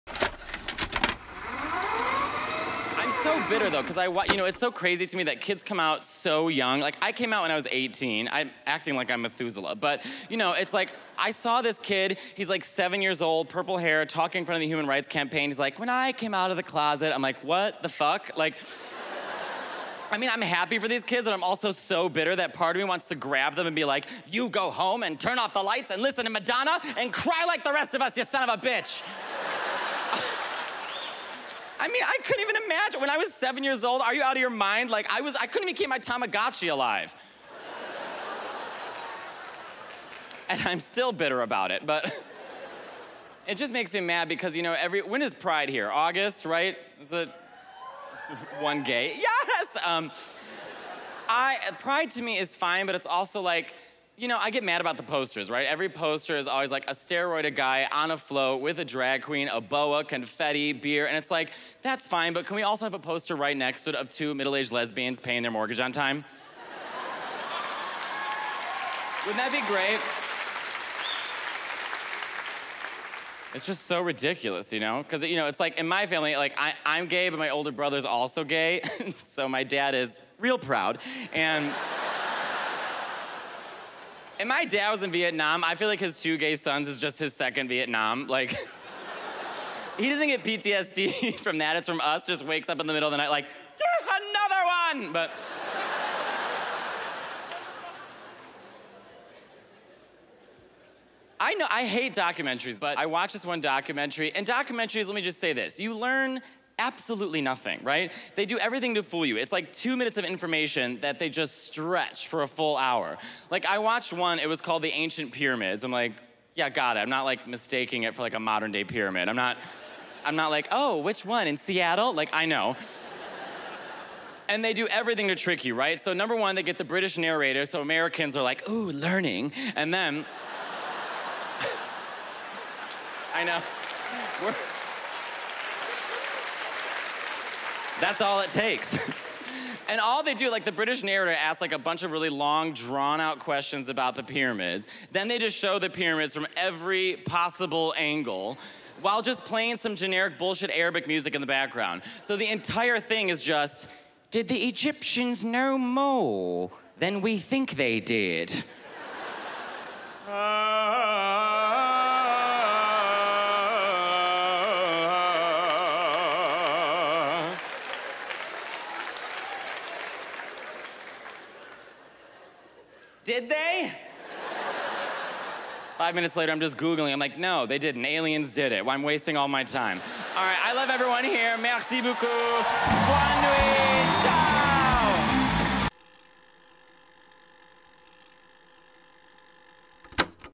Matteo Lane - All Documentaries Are Trying to Trick You
MatteoLane on how advanced kids are now, unrealistic Pride posters, and his hatred of documentaries.

MatteoLaneStandUp from the JustForLaughs Festival in 2018.